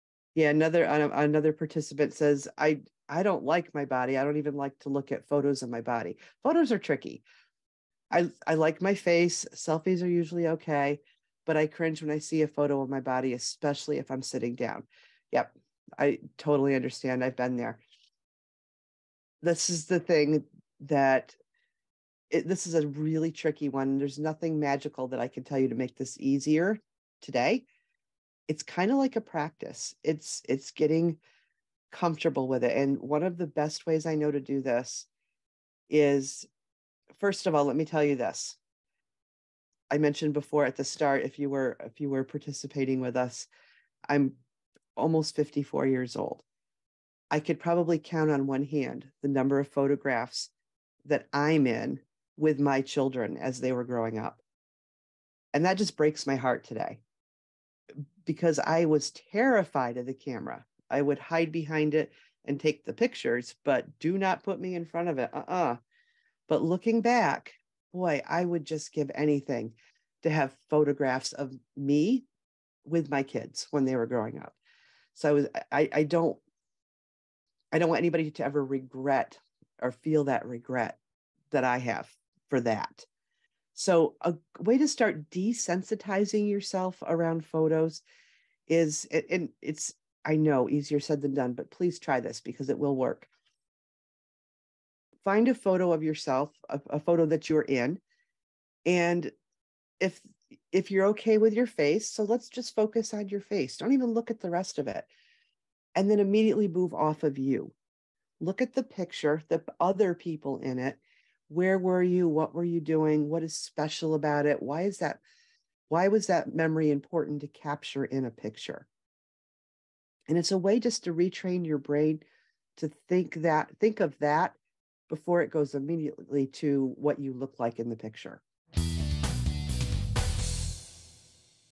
Let's Coach 2023 - Norma Frahn, Life Coach & Integrative Nutrition Coach
In one of the Let's Coach 2023 session, a participant types in "I don't like my body. I don't even like to look at photos of me. I cringe to see a photo of me, espcially if I'm sitting down." Norma Frahn,  Life Coach & Integrative Nutrition Coach responds with a personal story and invites us to start this practice around photos.